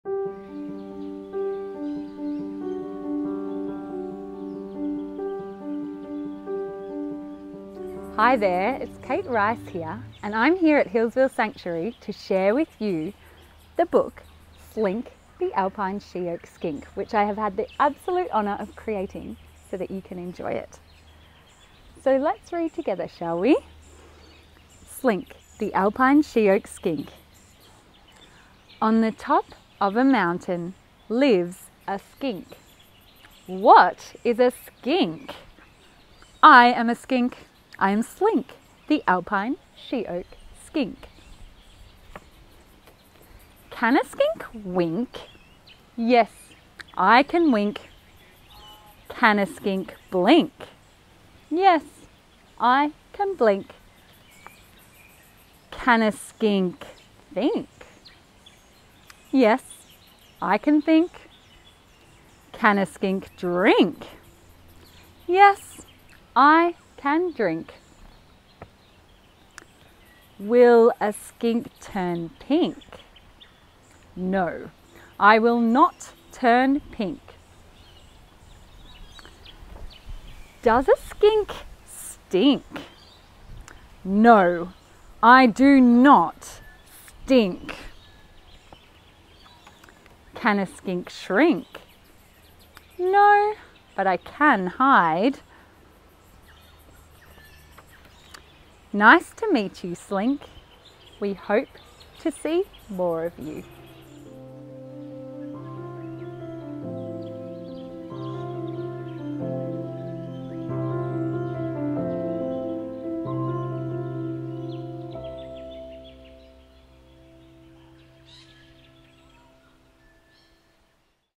Storytime: Slink the Alpine She-oak Skink
Hear author illustrator Kate Rijs read Slink the Alpine She-oak Skink, a story especially for Foundation-Year 2 students.

eBooks and teaching resources for Zoos Victoria's School Readers can be found